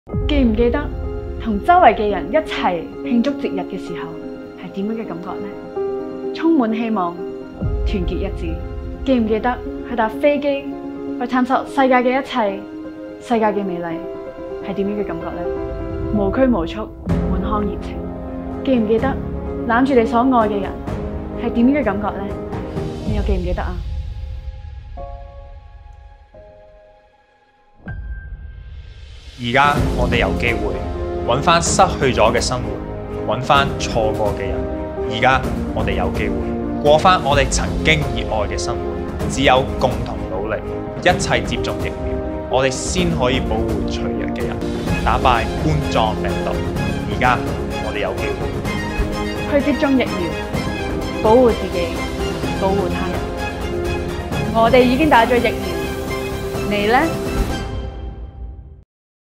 我們都接種了！
為鼓勵年輕人接種新冠疫苗，本會實習生陳愷喬（HKU MBBS）及天龍（HKU BSc）參與拍攝兩段短片，於七、八月期間，在本會社交媒體平台播放。多多支持！